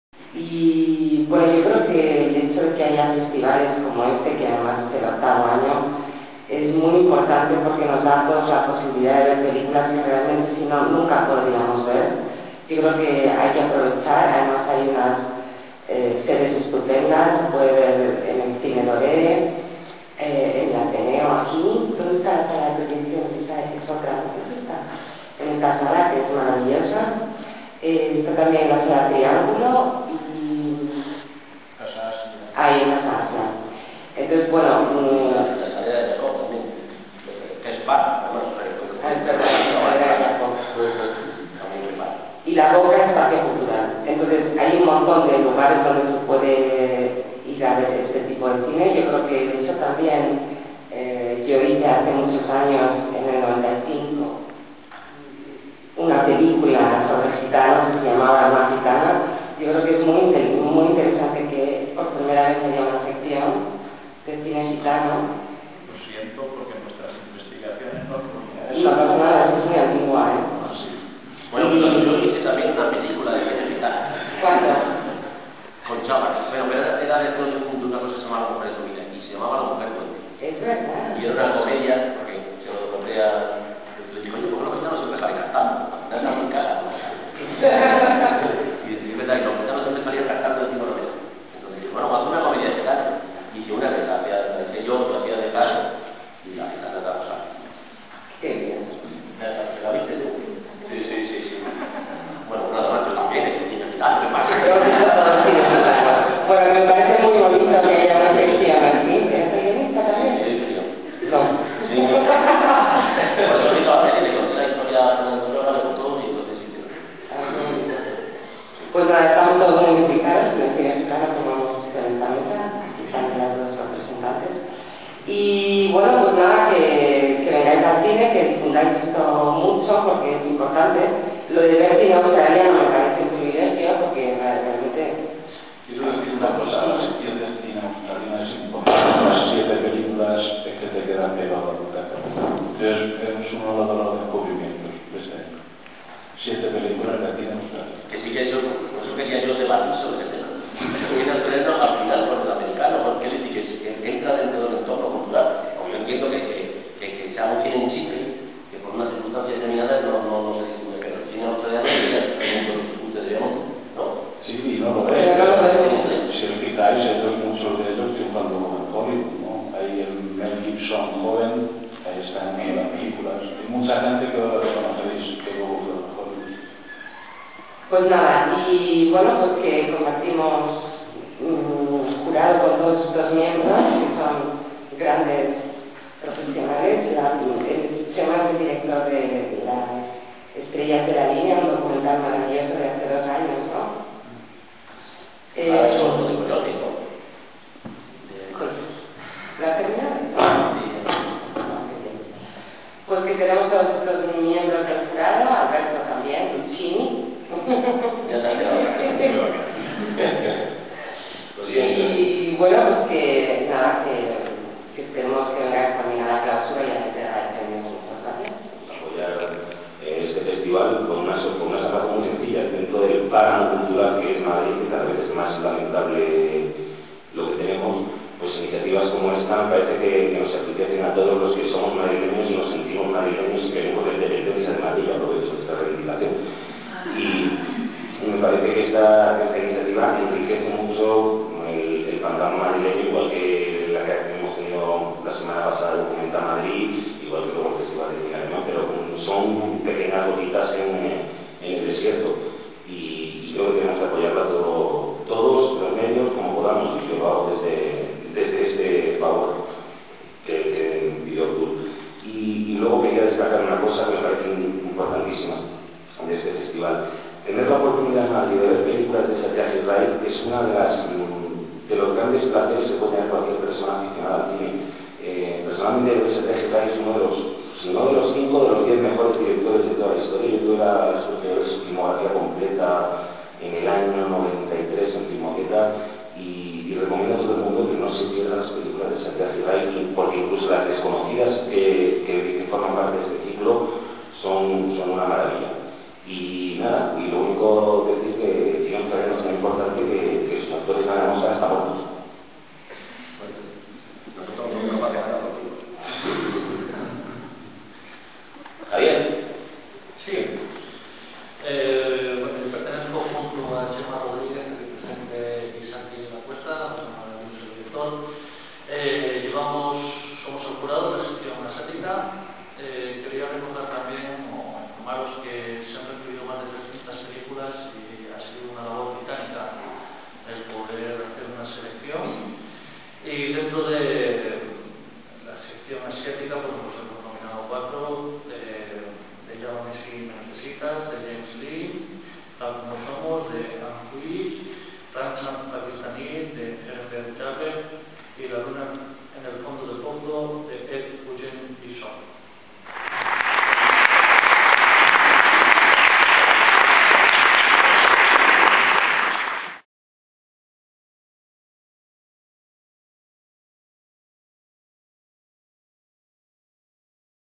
IMAGINEINDIA rueda de prensa
Rueda de prensa de ImagineIndia .Chus Gutierrez es jurado del Festival de Cine de India en Madrid